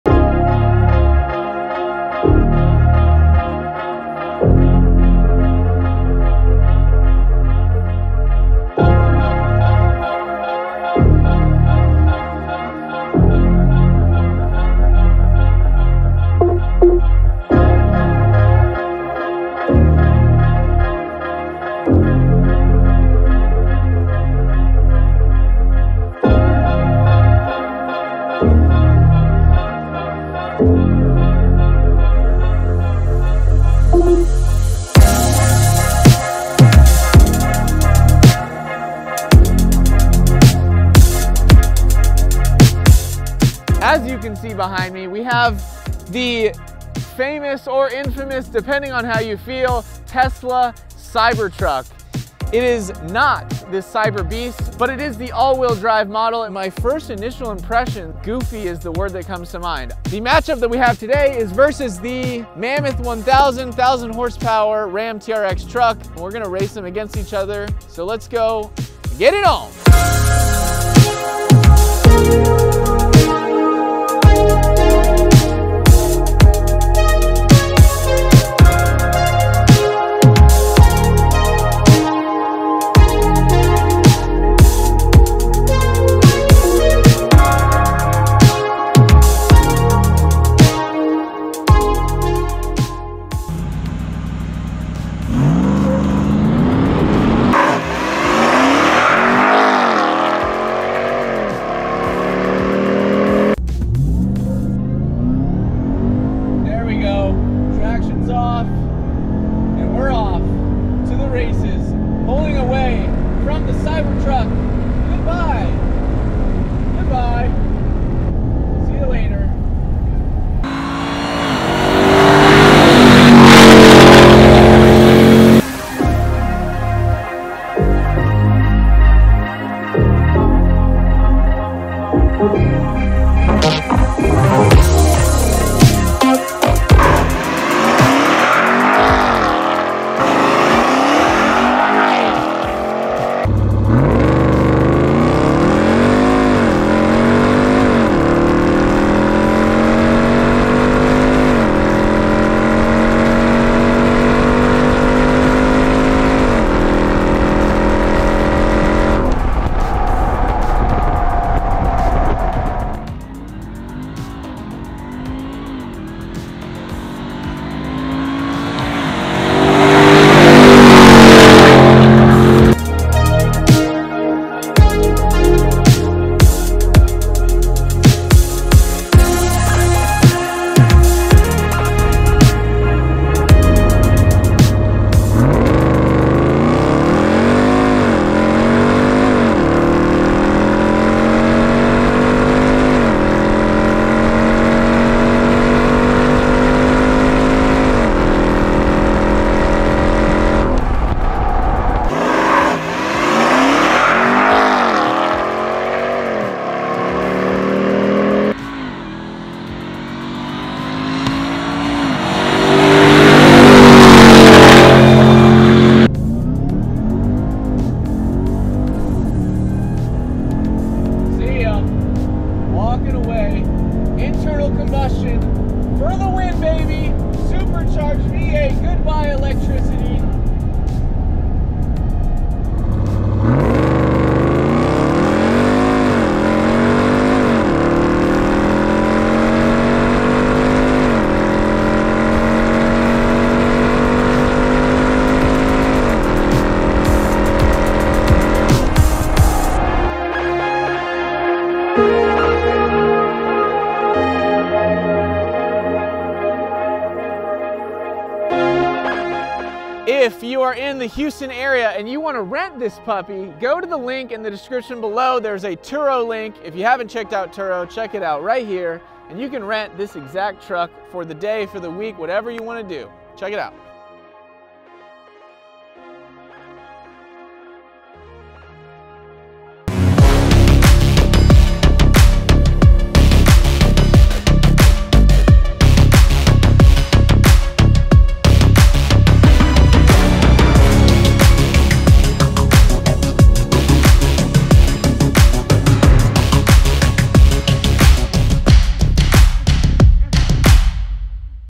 Cybertruck vs. 1000HP TRX Drag Race // Which is Faster, ICE or EV?
Buckle up for the race of the year! We got our hands on the HIGHLY anticipated @tesla Cybertruck and headed to the Pennzoil Proving Ground with our legendary MAMMOTH 1000 RAM TRX. This is NOT the CyberBeast but it is the AWD 'Foundation' Series Cybertruck with dual motors. The two trucks tip the scale at nearly the same gross weight and have been regarded as the best pickup trucks available on the market.

Interested in renting a Cybertruck in the Houston area? Drive it today

Learn all about the Hennessey MAMMOTH 1000 and our upgrades for the entire Dodge and RAM lineup on our